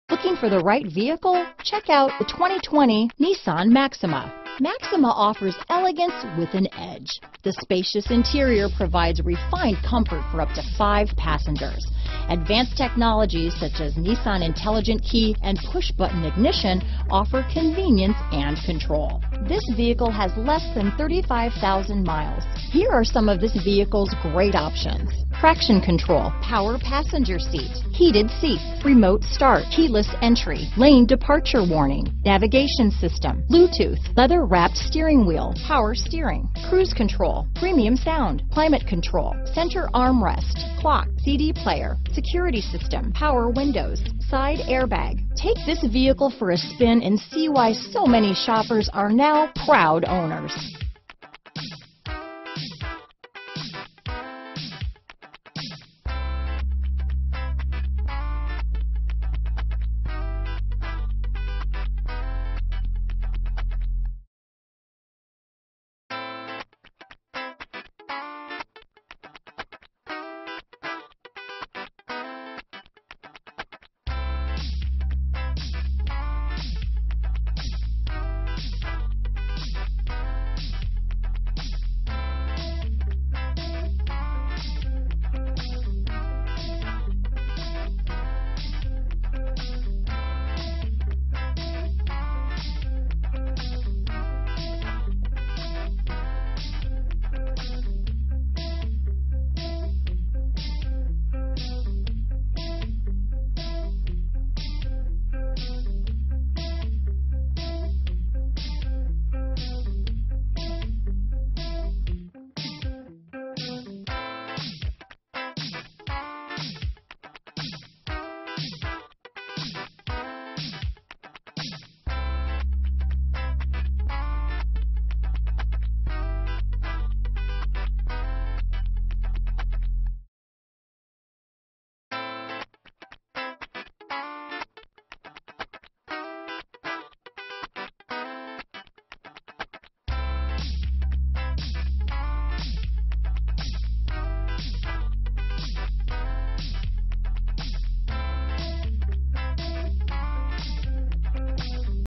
2020 Nissan Maxima Gainesville, Ocala, Lake City, Jacksonville, St Augustine, FL 1389
Deep Blue Pearl  2020 Nissan Maxima available in Gainesville, Florida at Tomlinson Motor Co. Servicing the Ocala, Lake City, Jacksonville, St Augustine, FL area.